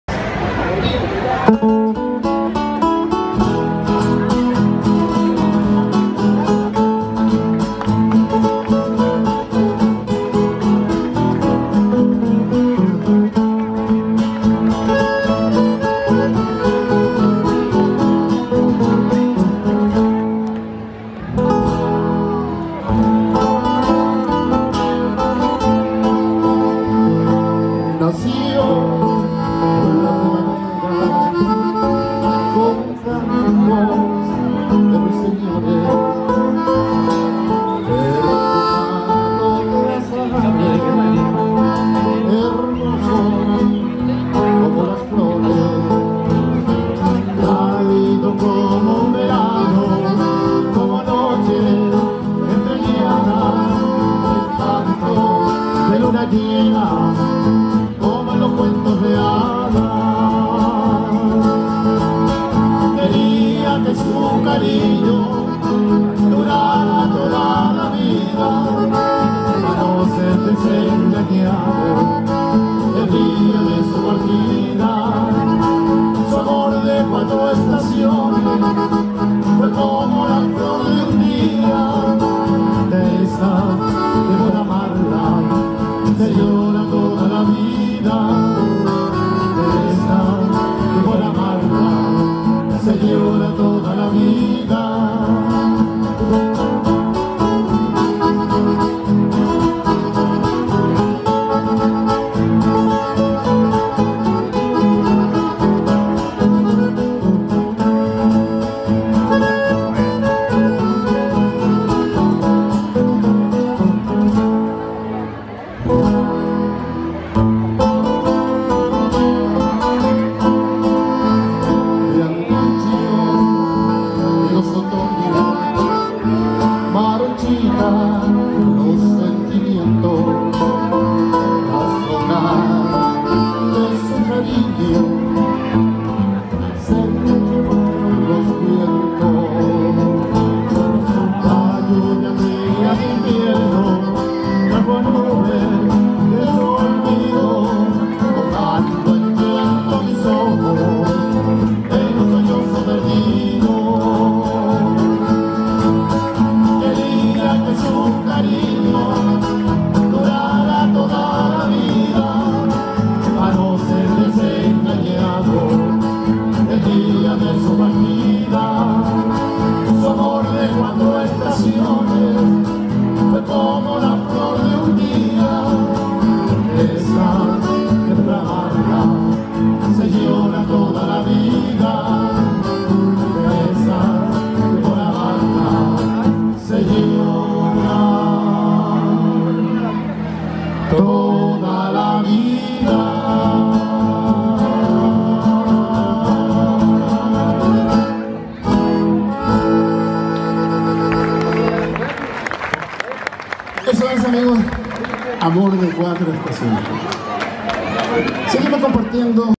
VENDIMIA 2019..santa cruz...Huasos corraleros...
VIDEOS DE LA FIESTA DE LA VENDIMIA..2019.CONJUNTOS:RENACER DE COLCHAGUA,LAS COLCHAGUINAS,HUASOS CORRALEROS,VOCES DE ANTAÑO,KAMAC,CONJUNTO SANTA CRUZ,GRUPO CHAMANTO.